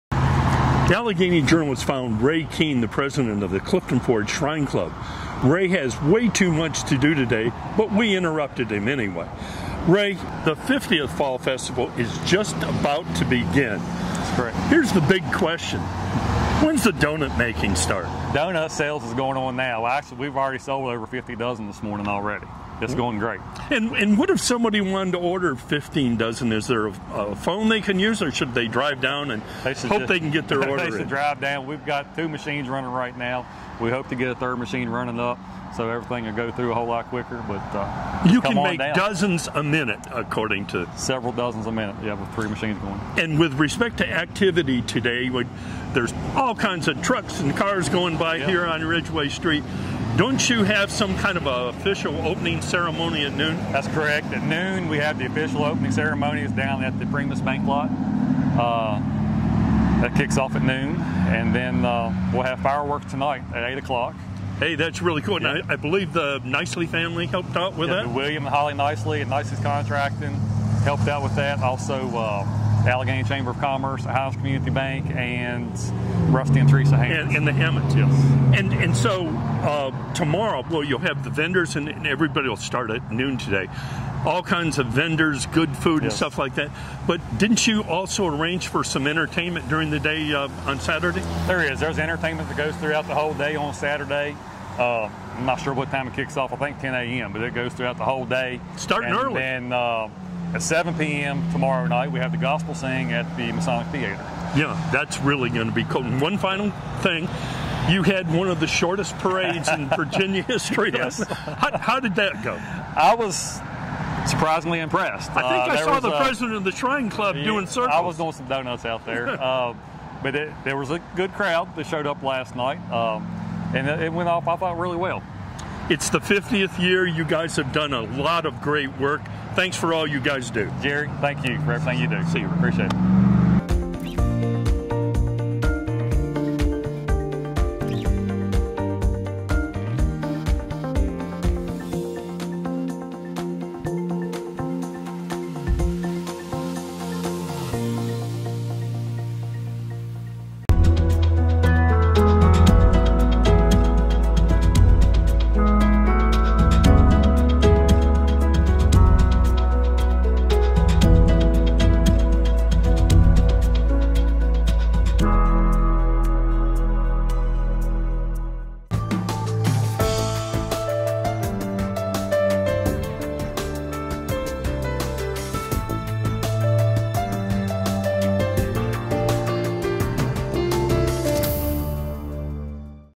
CF Shrine Club President: Lots Of Action Happening At 50th Fall Foliage Festival
The Alleghany Journal found Clifton Forge Shrine Club president Ray Keen on Ridgeway St. in Clifton Forge this morning - with cars and trucks buzzing by and lots of activity going on. The donut machines are running full tilt, Keen said. Lots of entertainment is planned for the weekend, many vendors and food outlets, plus some fireworks this evening (Friday, Oct. 15, 2021), courtesy of some local donors.